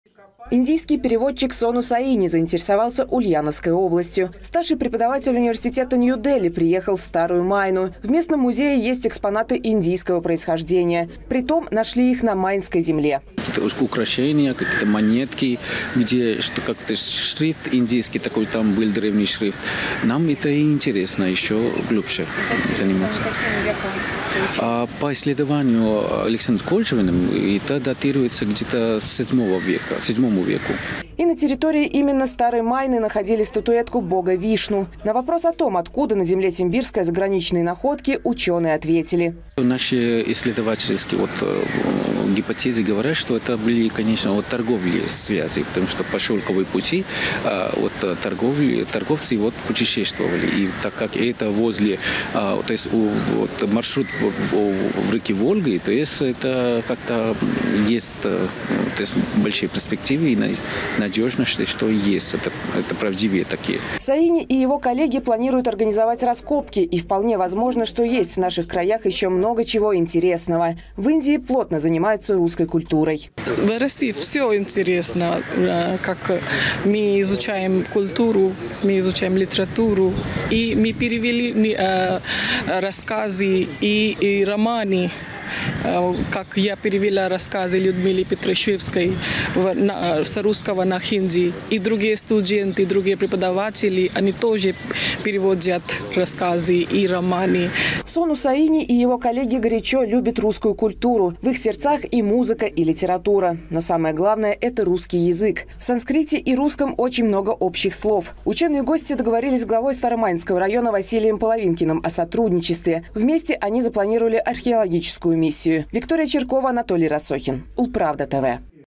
Из Индии - в Старую Майну. Зачем в Ульяновскую область пожаловали заграничные гости
Россию можно бояться, можно и любить. Но точно огромная страна с богатой культурой не оставляет никого равнодушным. Давний и древний друг, космическая держава Индия тесно связана с нашей страной. И научные деятели проводят совместные исследования, двигают прогресс. Чем заинтересовала Старая Майна преподавателей уважаемого индийского университета – узнавала Виктория Чиркова.